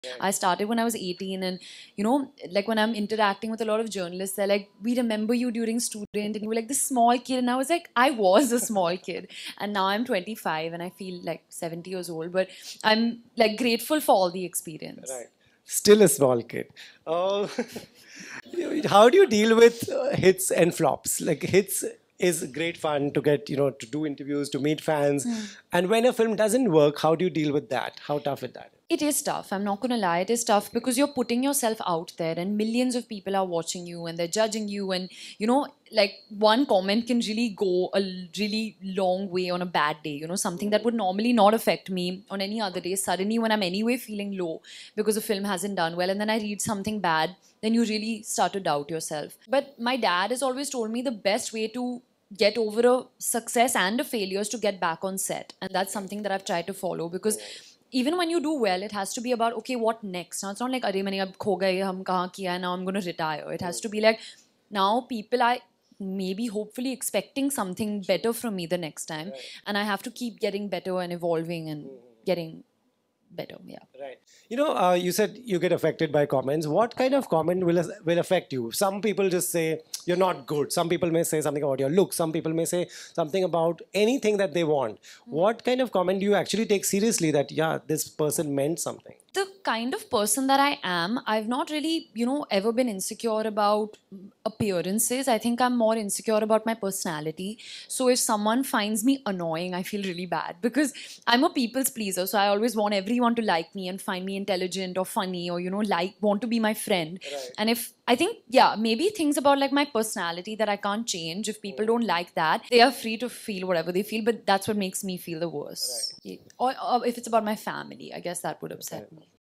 Dealing with Hits and Flops How I Overcome Success and Failure
I started when I was 18 and you know like when I am interacting with a lot of journalists they are like we remember you during student and you were like this small kid and I was like I was a small kid and now I am 25 and I feel like 70 years old but I am grateful for all the experience Still a small kid How do you deal with hits and flops like hits is great fun to get you know to do interviews to meet fans and when a film doesn't work how do you deal with that how tough is that It is tough I am not going to lie it is tough because you are putting yourself out there and millions of people are watching you and they are judging you and you know like one comment can really go a really long way on a bad day you know something that would normally not affect me on any other day suddenly when I am anyway feeling low because the film hasn't done well and then I read something bad then you really start to doubt yourself but my dad has always told me the best way to get over a success and a failure is to get back on set and that is something that I have tried to follow because even when you do well it has to be about okay what next now it is not like I have lost now where have I done now I am going to retire it has to be like now people are maybe hopefully expecting something better from me the next time and I have to keep getting better and evolving and getting better yeah You know you said you get affected by comments what kind of comment will affect you some people just say you are not good some people may say something about your looks some people may say something about anything that they want what kind of comment do you actually take seriously that yeah this person meant something The kind of person that I am I have not really you know ever been insecure about appearances I think I am more insecure about my personality so if someone finds me annoying I feel really bad because I am a people's pleaser so I always want everyone to like me and find me intelligent or funny or you know like want to be my friend and if I think yeah maybe things about like my personality that I can't change if people don't like that they are free to feel whatever they feel but that's what makes me feel the worst or if it's about my family I guess that would upset me